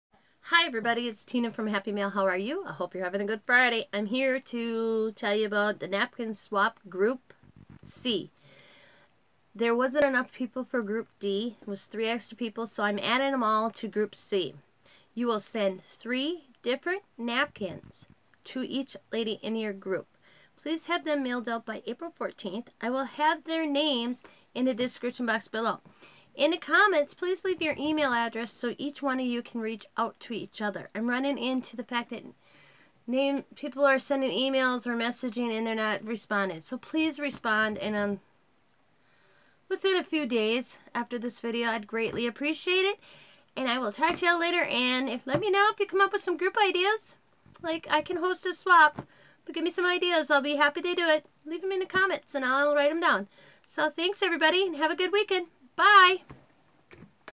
NAPKIN SWAP Group C names in description below
Please leave your email in the comments so that you ladies can get each other address , please have mail out by April-14 'Thank You
One Group C 13 people
Carolyn Sitton
Patti Jo Sinerius
Debbie Hughes
Pink Butterfly
Crafting with Aloha Cherie
Crafts and more with Donna Jackson
Leticia Lightfoot
Jennifer Watts
Happy Mail Swapper
Deborah Sweeny
New Scrapper/Diva's Creation
Marvala Allen
Kimzeb 0410